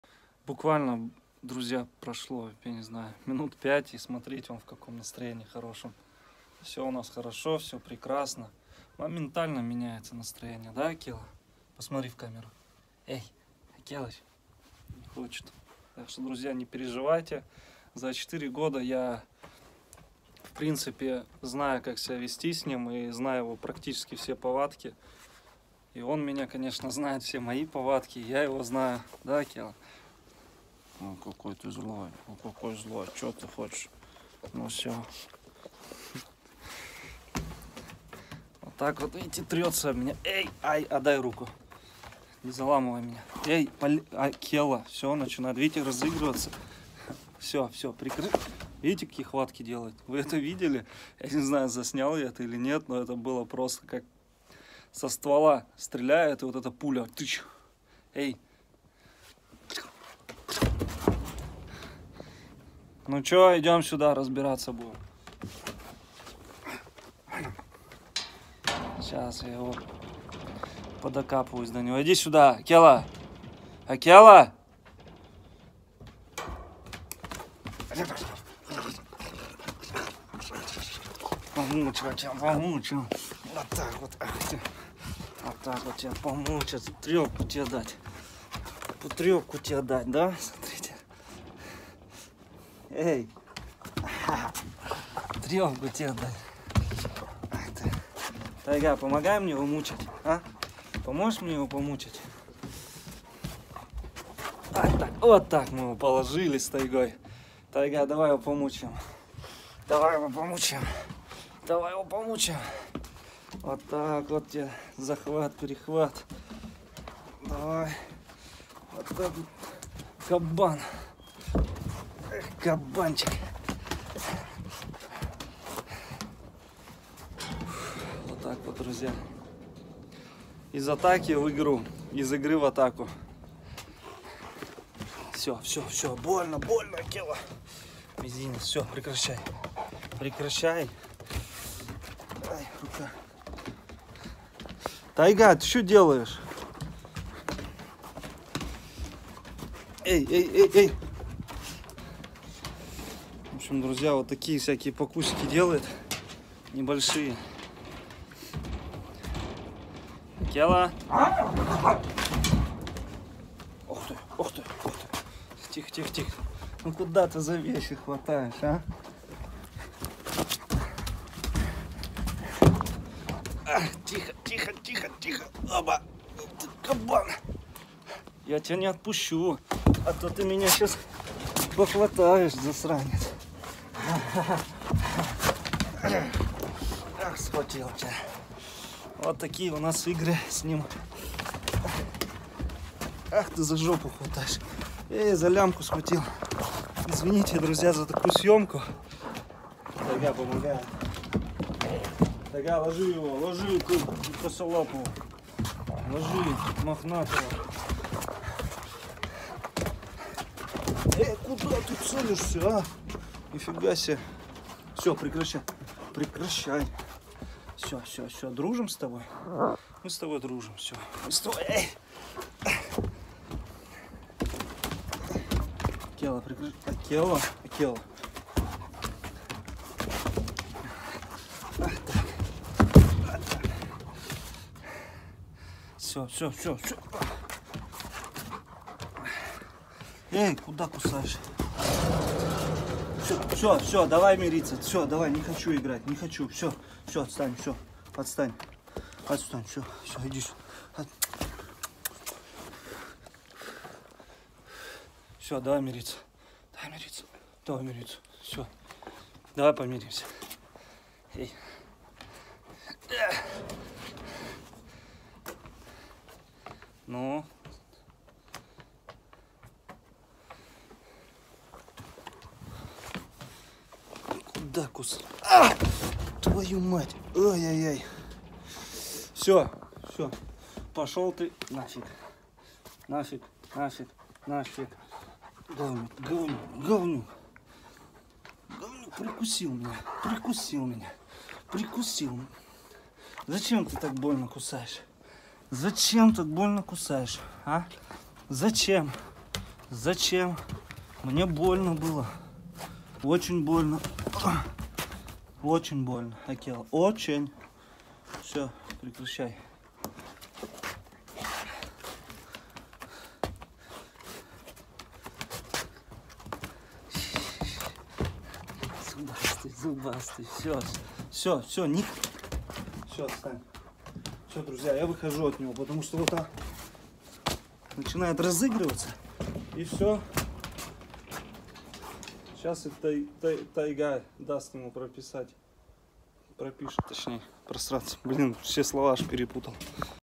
Укус ВОЛКА в игре очень болезненный, Канадский волк Акела играется
Волк в игре прикусывает меня и тем самым проверяет на прочность , малейший испуг и следующий укус будет режущий. Раны, синяки и гематомы заживают долго.

Даже 10 рублей, УСКОРИТ создание Проекта